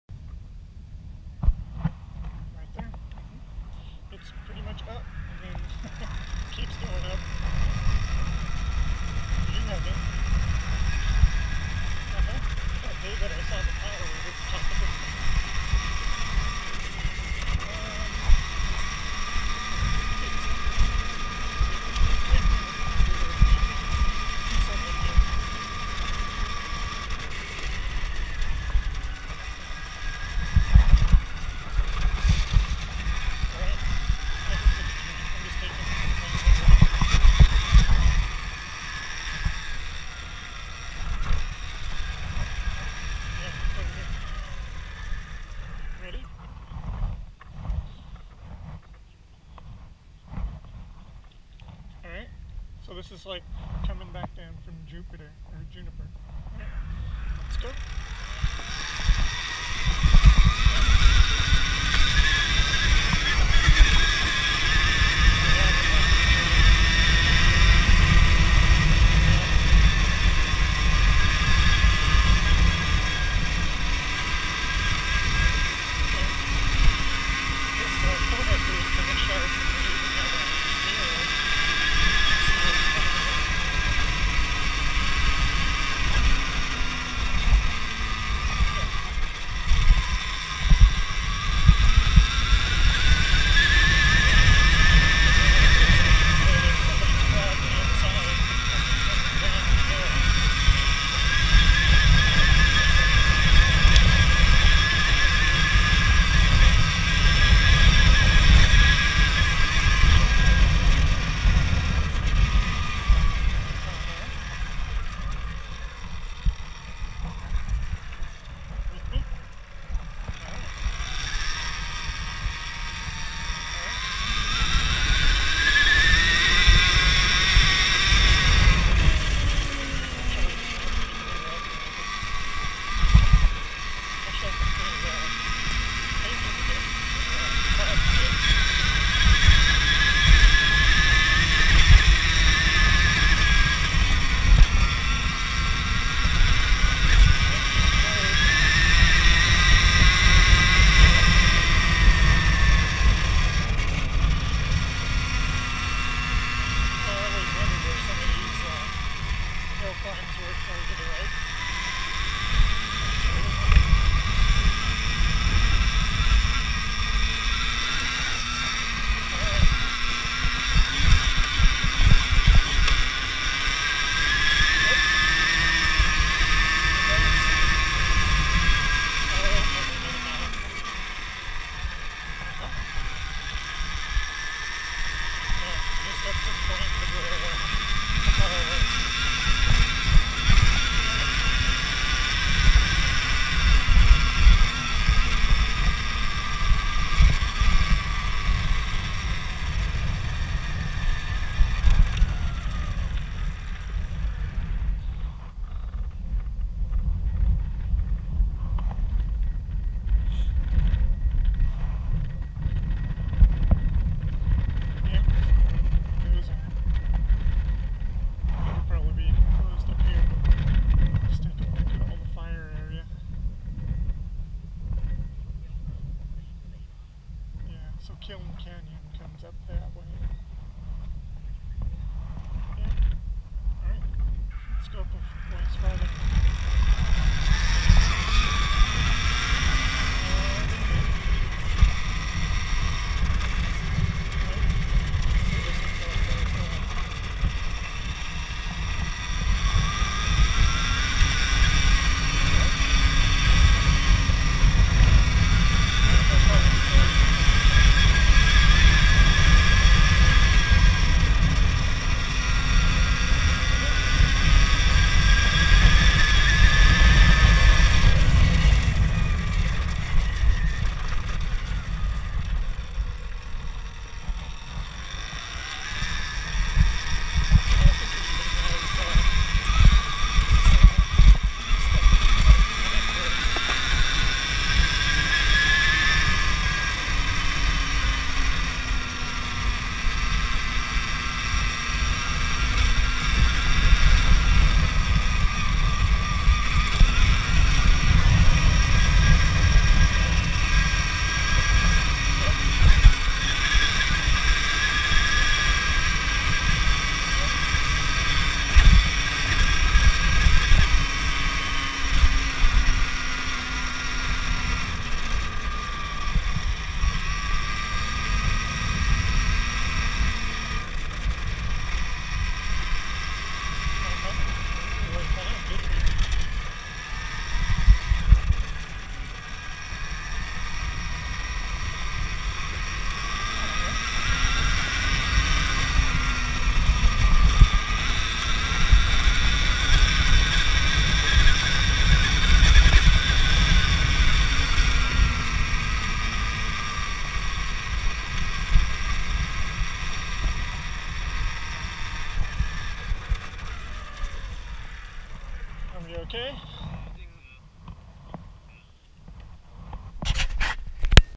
Riding Carnegie SRVA - Los Osos Trail - Adjacent to Hollow fire burn area
Riding Carnegie SRVA on my Alta Redshift MXR - Going up Los Osos Trail.  This is adjacent to Hollow Fire burn area, which hit the park a few weeks ago.  The area will be closed indefinitely while this part of the SRV park is rehabbed.

Riding with my son on his Sur-Ron Light Bee X.   (By the way, we have wireless intercoms, so I'm not really mumbling to myself in these vids!)

Camera:  GoPro Hero4 - Chest mount
Intercoms:  Sena 20s
Bikes:  2018 Alta Redshift MXR  & Sur-Ron Light Bee X.